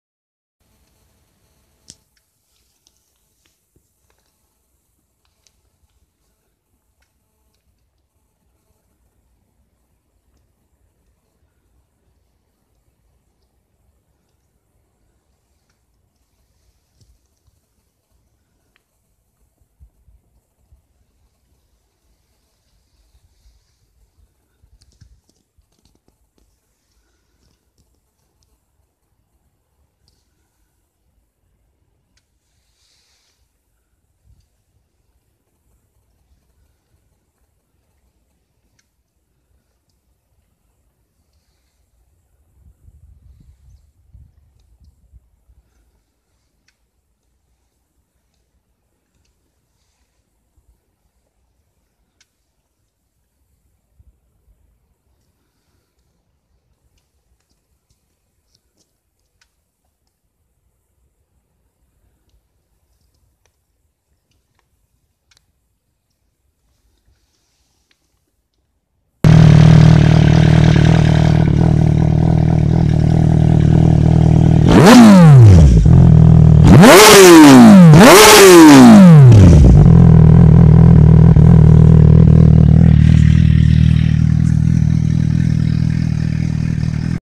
Custom tuning Suzuki GSR 600 Virus Streetfighter
After few months of work,there is the result. My brand new motorcycle.